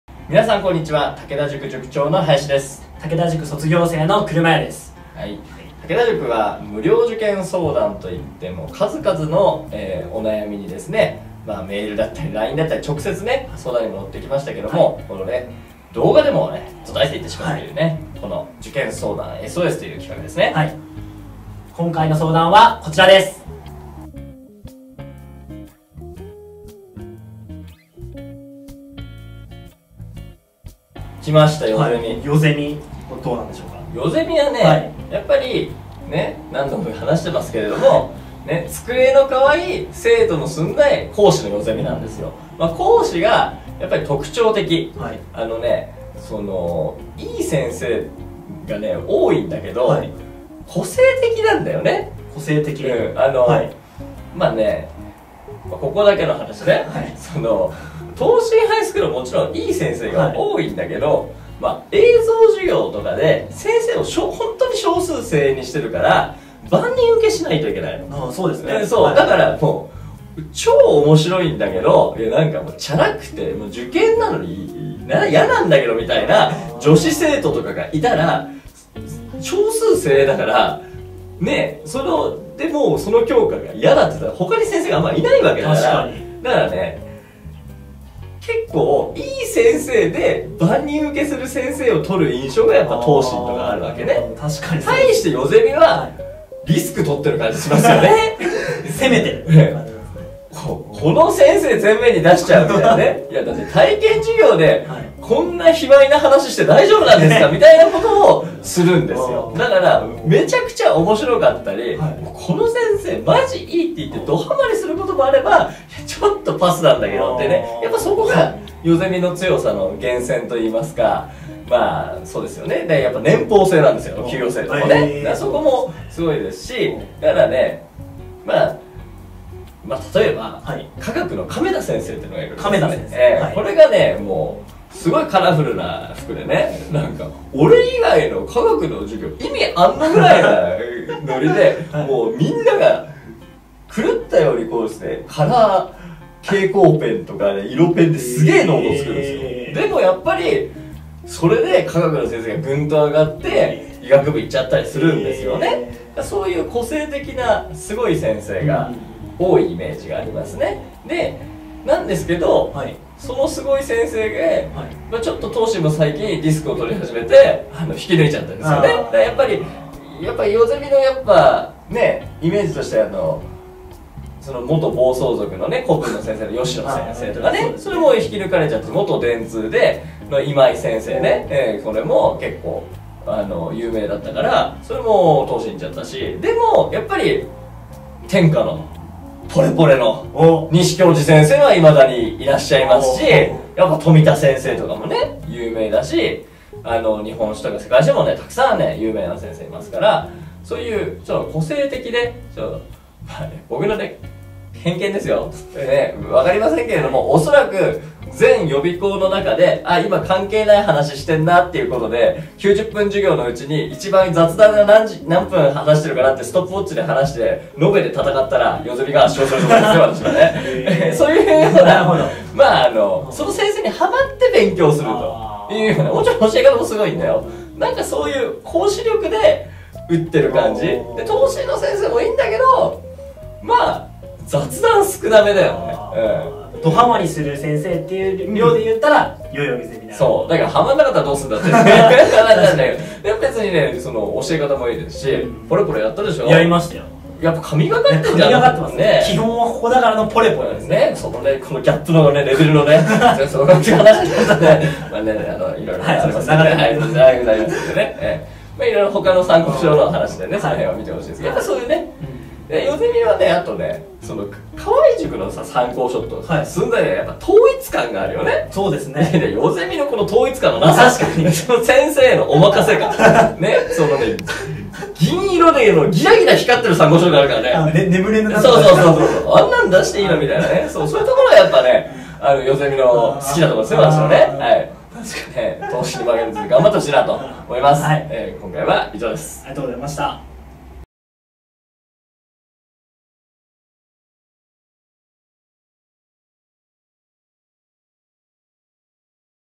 代ゼミ仙台校の評判や口コミ、合格実績｜受験相談SOS
武田塾があなたの受験の疑問にお答えする【受験相談SOS】のコーナー。
代ゼミ仙台校の評判や口コミ、合格実績に関する質問にお答えしました。
河合塾や駿台と並んで、屈指の予備校代々木ゼミナール。
その代ゼミの仙台校とはどんな校舎なのでしょうか。
代ゼミ仙台校に入塾を考えている受験生は必見です！
センター試験 古文・漢文の点数が面白いほど取れる問題集の使い方と評判